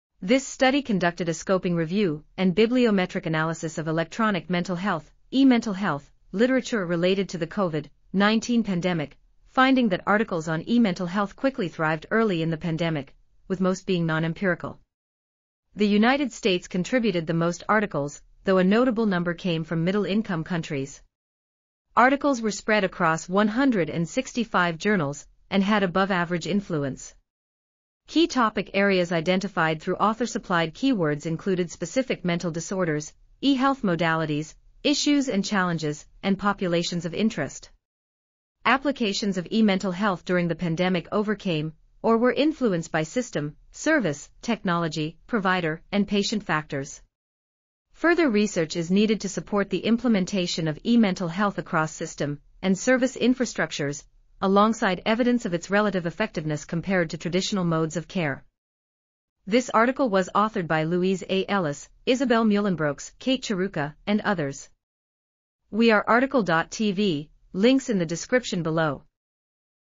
The Application of e-Mental Health in Response to COVID-19: Scoping Review and Biblio... | RTCL.TV
### Keywords ###

### Article Attribution ###
Title: The Application of e-Mental Health in Response to COVID-19: Scoping Review and Bibliometric Analysis
Authors: Louise A Ellis, Isabelle Meulenbroeks, Kate Churruca, Chiara Pomare, Sarah Hatem, Reema Harrison, Yvonne Zurynski ,and Jeffrey Braithwaite

### Video Timestamps ###
0:00:00 - Summary
0:00:59 - Title
0:01:05 - Tail
0:01:09 - End